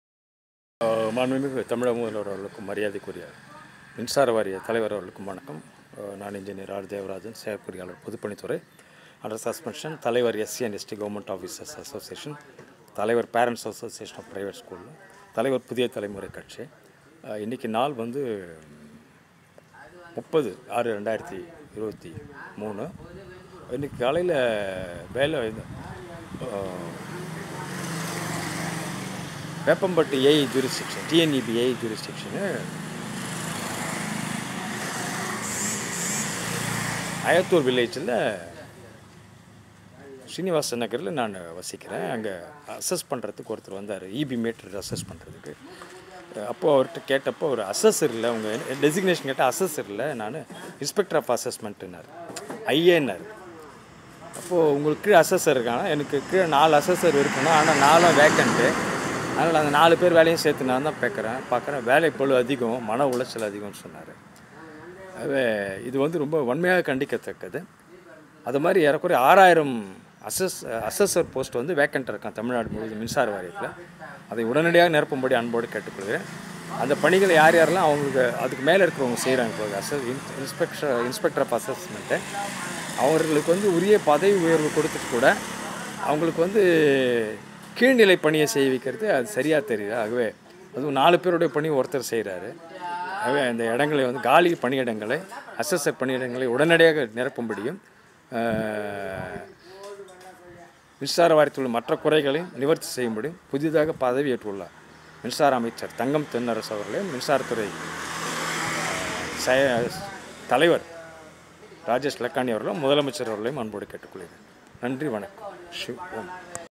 TNEB has to fill Assesser posts immediately.
Voice of Swamy Devarajan the President NGP dt.30 06 2023.